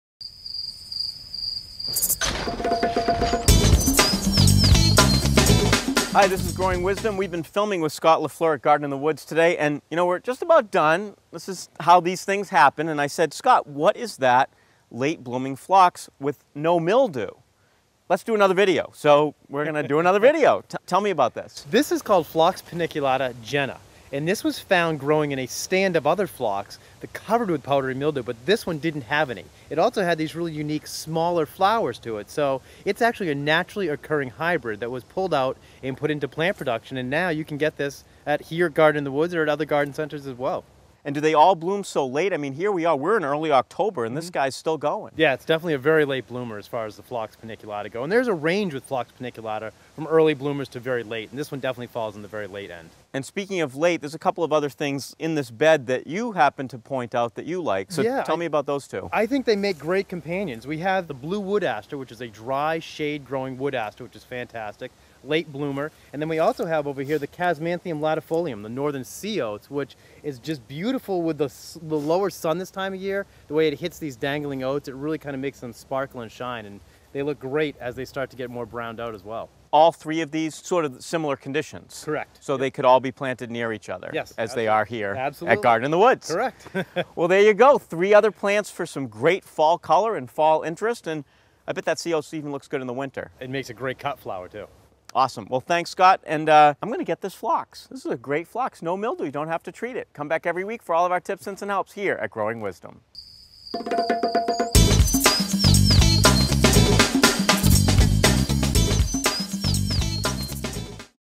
Learning About Fall-Blooming Native Plants
Dave talks with Scott LaFleur, botanical director of Garden In The Woods, about three different fall-blooming native plants, including a mildew-resistant phlox.